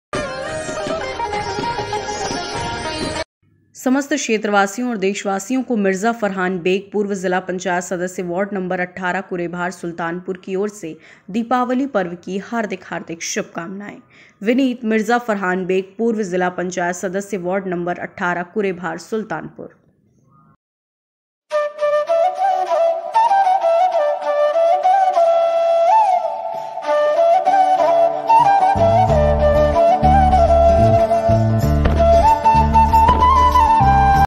समस्त क्षेत्रवासियो को मिर्जा फरहान बेग , पूर्व जिला पंचायत सदस्य की ओर से दीपावली की शुभकामनाएं
समस्त क्षेत्रवासियो एवं देशवासियों को मिर्जा फरहान बेग , पूर्व जिला पंचायत सदस्य वार्ड नंबर 18 कूरेभार सुल्तानपुर की ओर से दीपावली पर्व की हार्दिक हार्दिक शुभकामनाएं
विनीत - मिर्जा फरहान बेग ,पूर्व जिला पंचायत सदस्य वार्ड नंबर 18 कूरेभार सुल्तानपुर